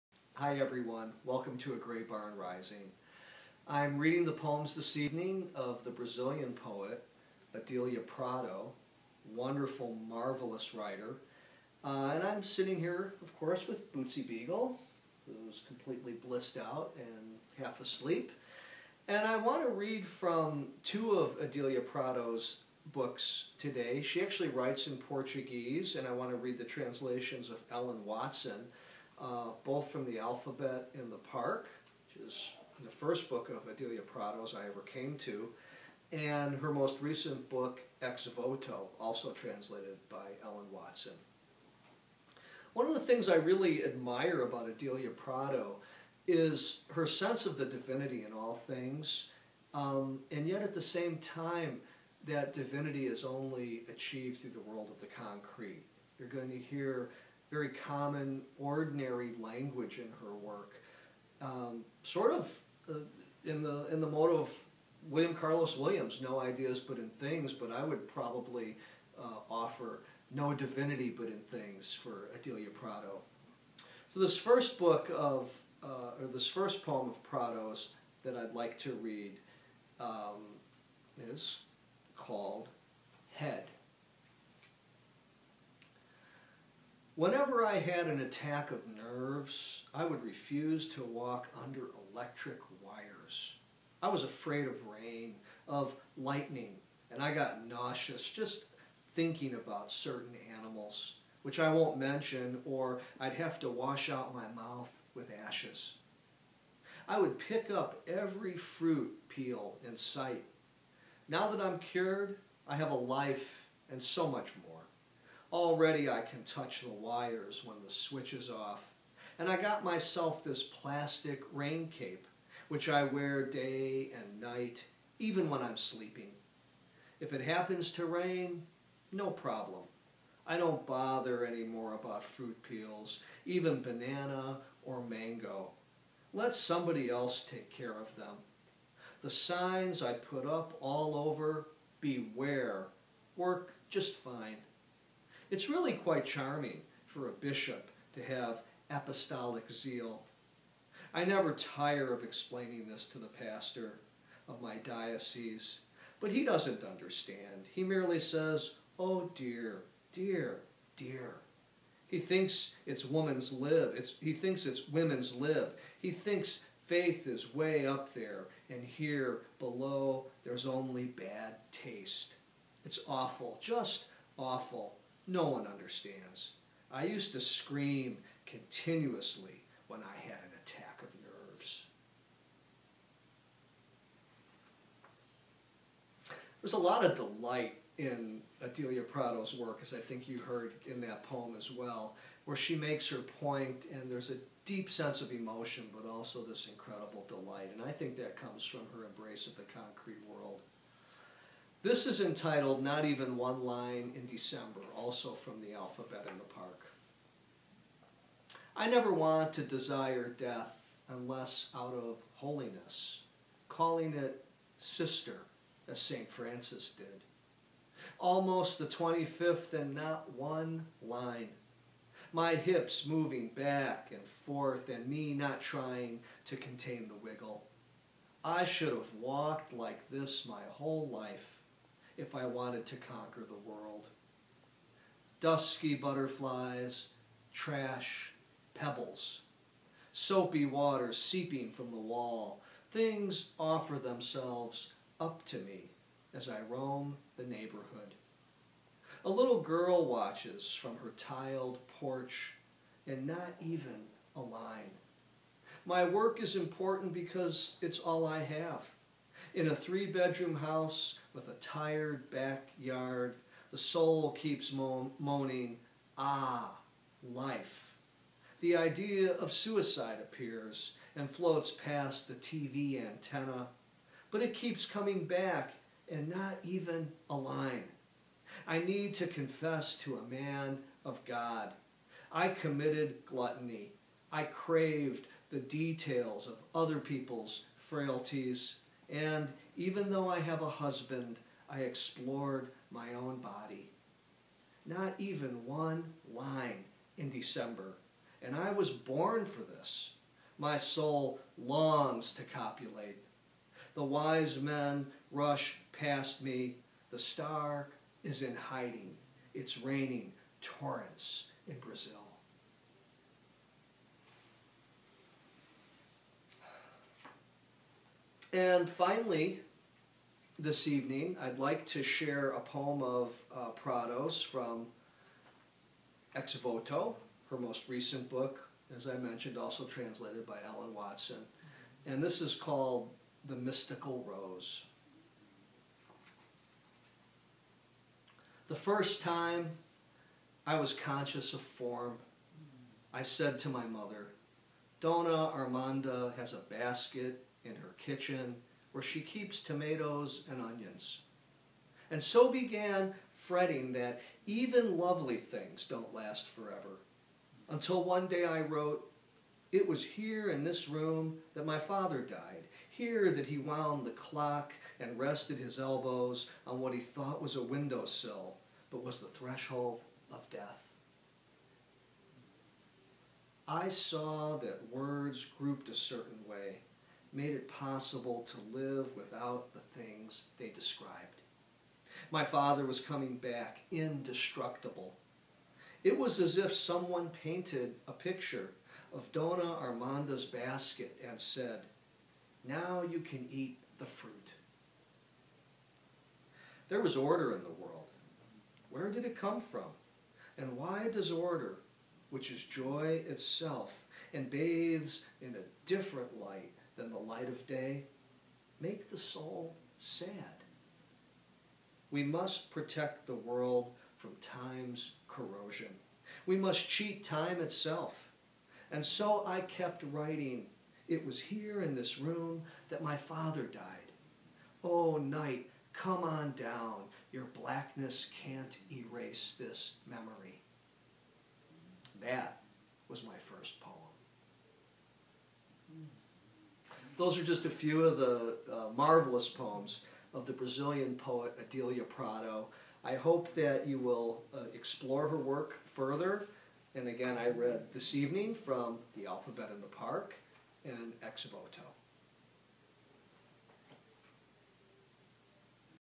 A Gray Barn Rising: Adélia Prado
Video Blog of the Poet Laureate of Indiana

In this installment, George reads and comments on the work of poet Adélia Prado.